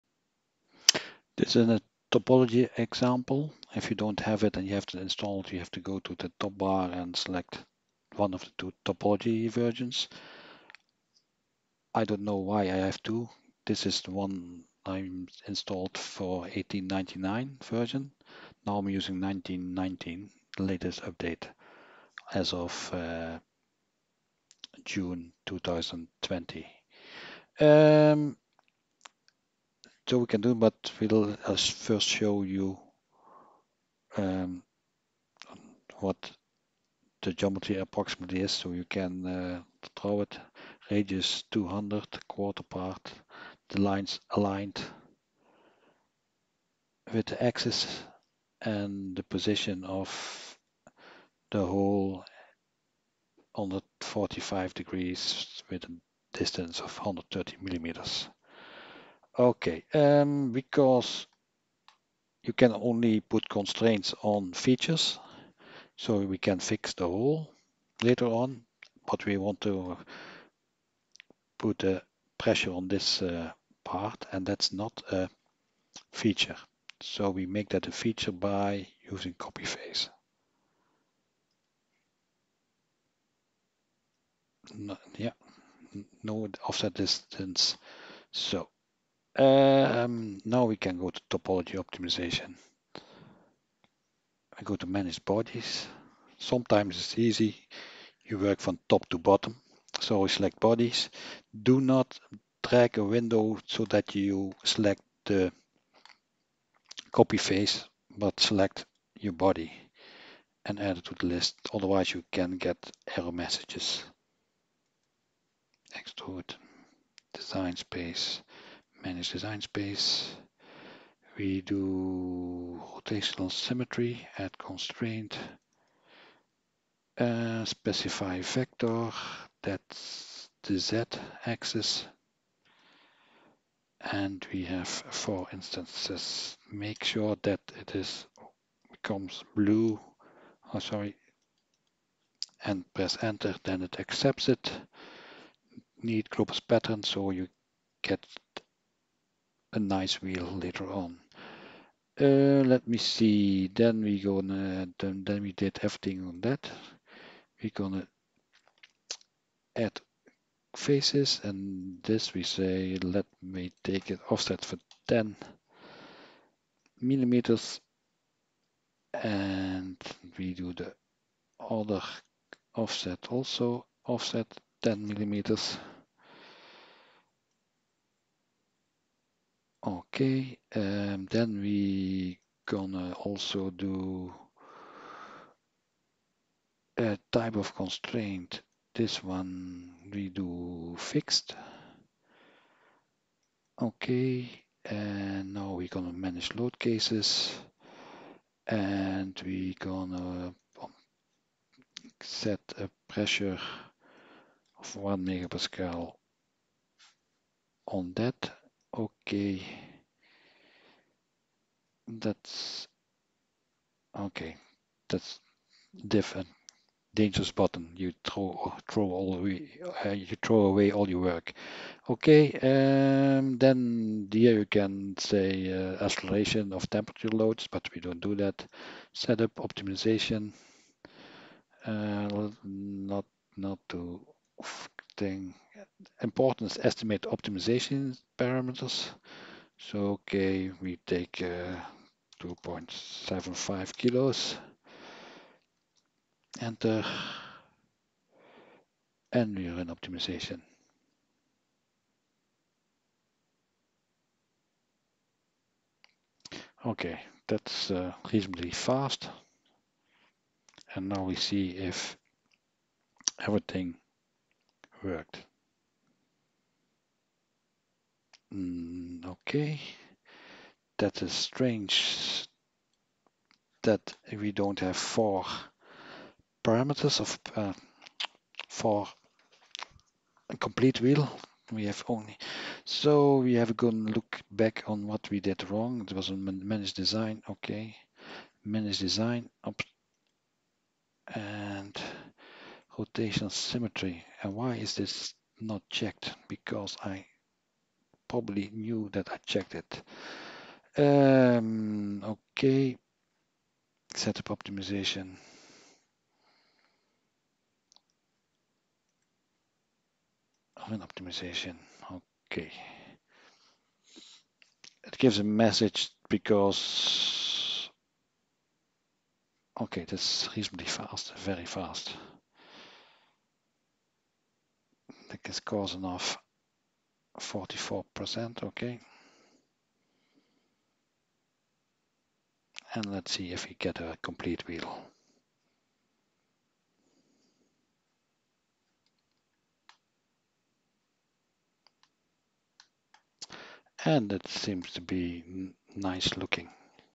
How to use rotational symmetry in Siemens NX topology optimization, and some error advises
How to use rotational symmetry in Siemens NX topology optimization, and some error advises when the result is not rotationally symmetric. An example is done in Siemens NX version 1919 but the user interface is identical in NX12 and upward.